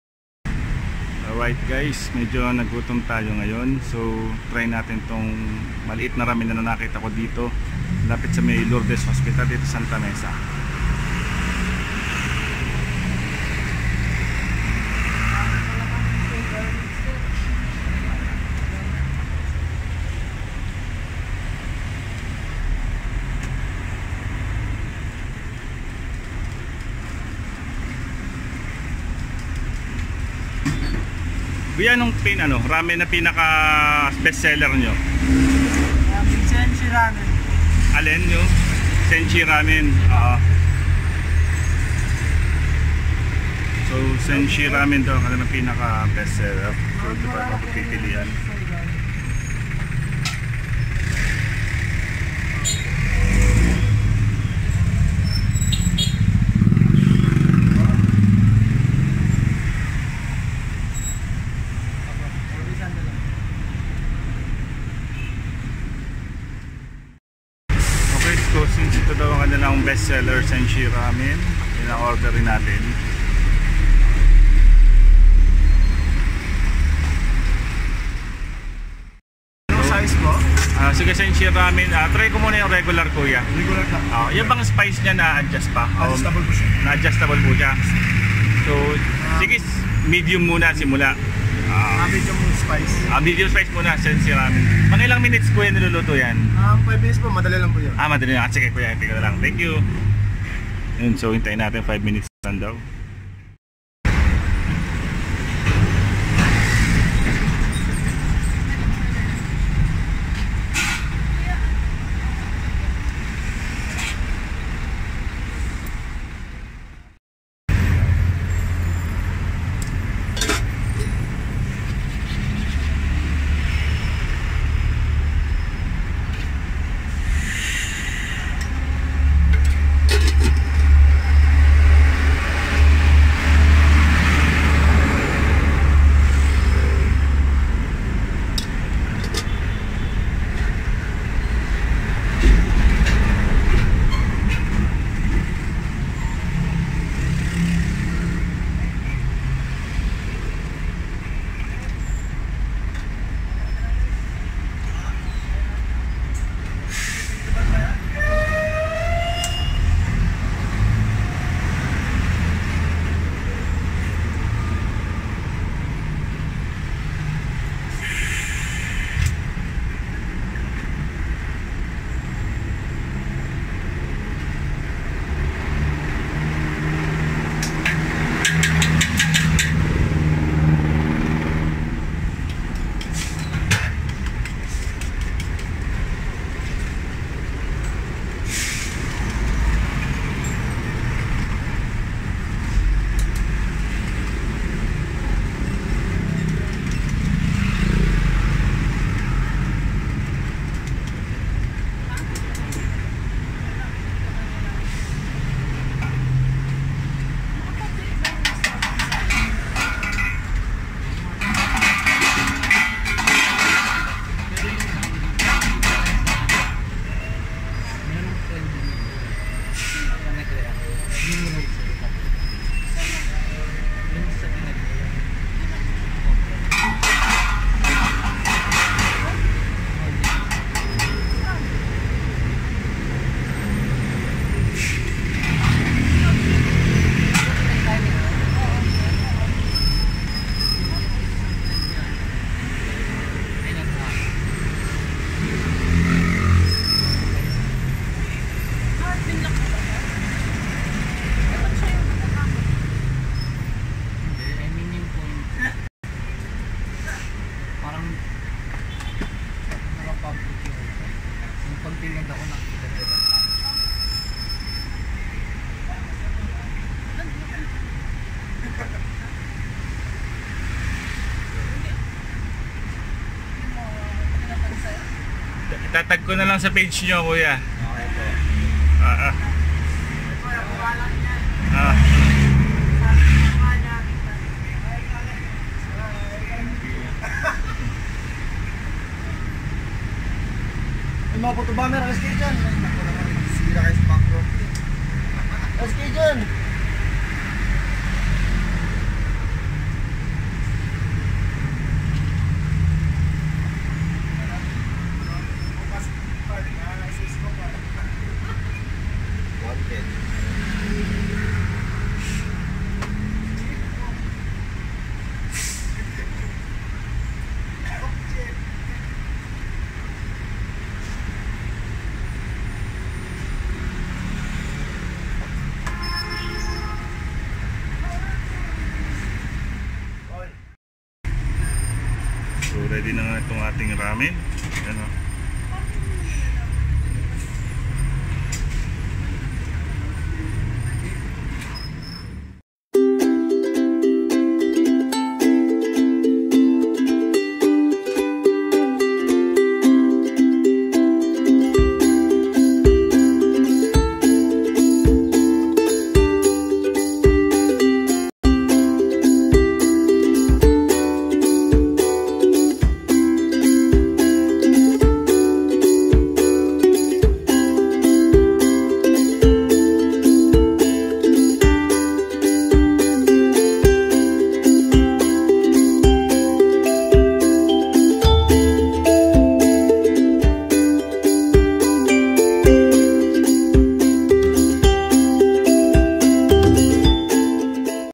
MURA AT MASARAP NA RAMEN SA MAY STA MESA MANILA
Sa mga budget meal jan na tipong gusto mag ramen, sulit dito sa Yakitori. Mura na masarap pa.